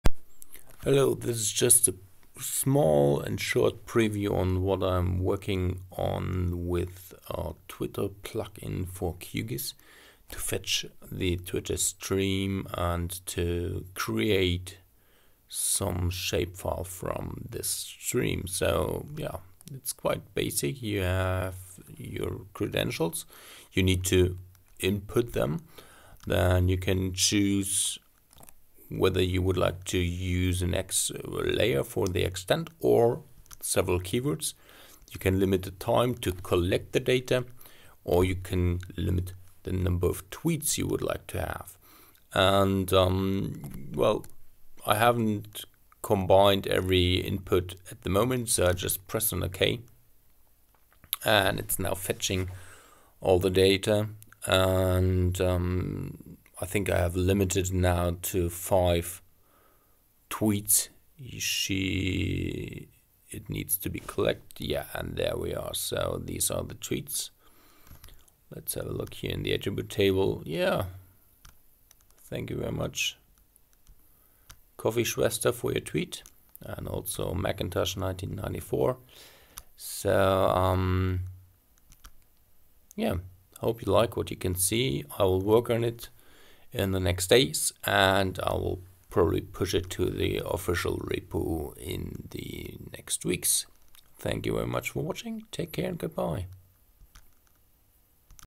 geotweet preview
a short preview: my new twitter plugin to collect twitter data as shapefiles